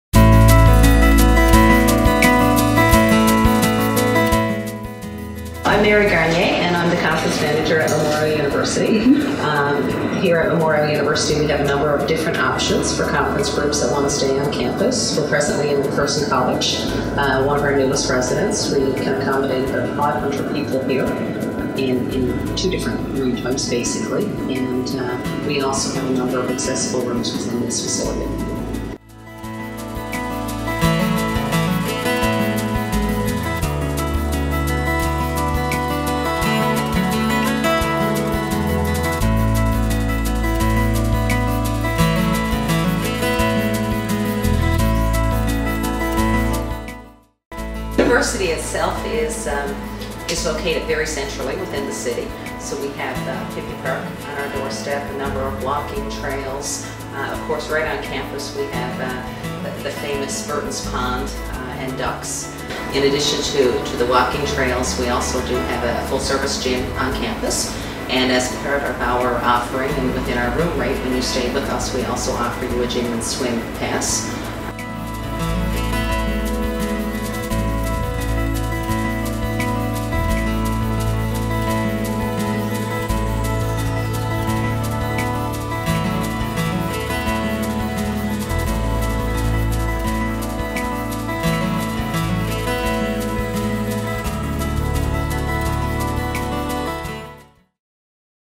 Memorial University Conferences & Summer Accommodations
Beyond university accommodation and conference expectations.
Memorial University of Newfoundland's Conferences & Summer Accommodations offer:
• Capacity for nearly 2,000
• Variety of room options
• Fully Accessible Rooms and Facilities
• Wireless Internet Throughout Campus
• Full Conference and Breakout Capability
• Tunnel System Allows Indoor Connections Between Buildings
• Gym & Swim Pass
• City Centre: Easy Walk, Bike or Bus Ride throughout St. John's
• Friendly, welcoming staff